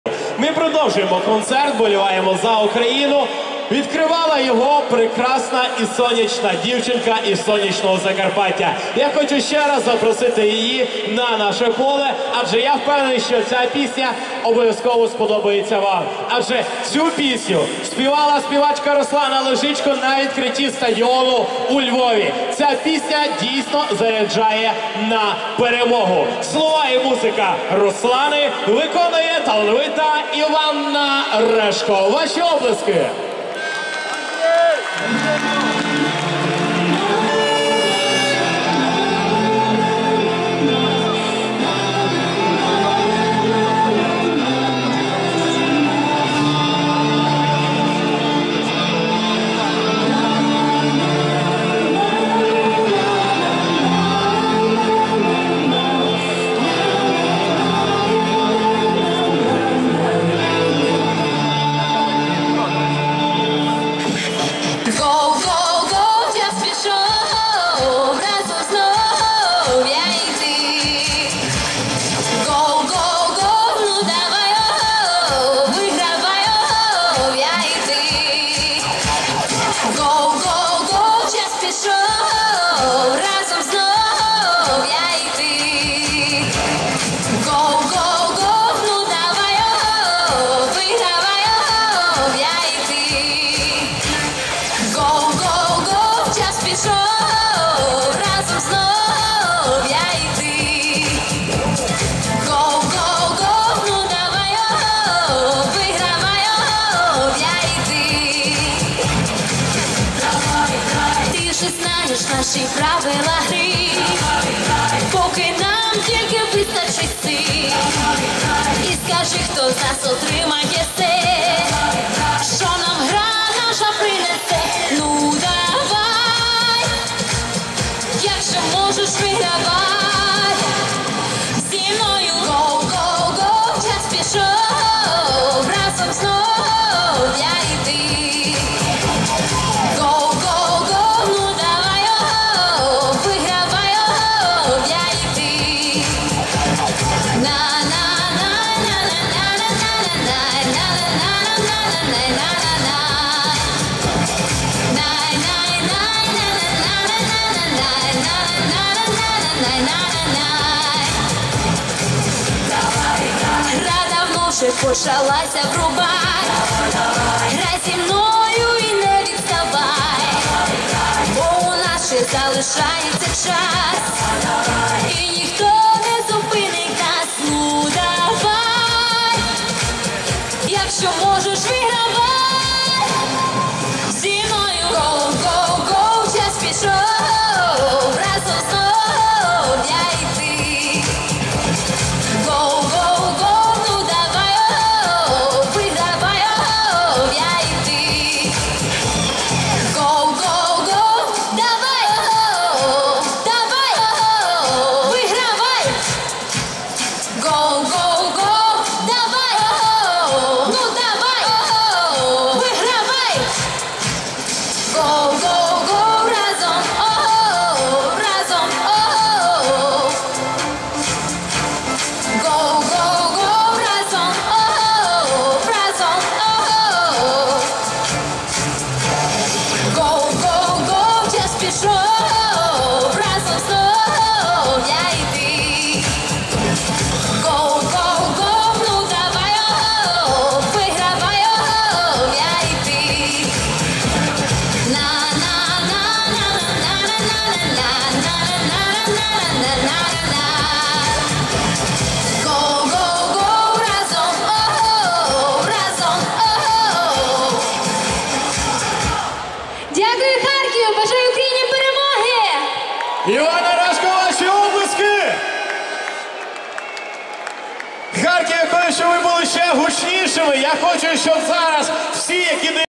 Іванна Решко.2.09.2017
Концертна програма "Вболівай за Україну" на матчі з футболу Україна-Туреччина.